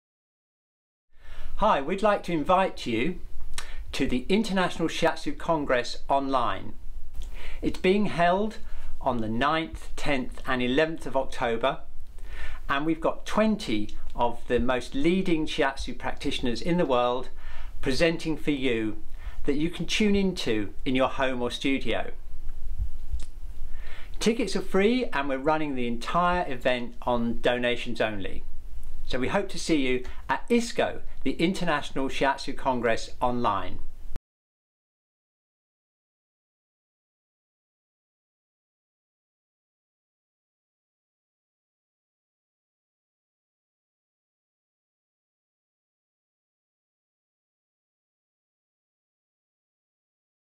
International Shiatsu Congress Online 9th 10th 11th October 2020
Please register now for your Free ticket to the International Shiatsu Congress on line Meet and learn from 20 of the world's leading Shiatsu teachers at your home or studio.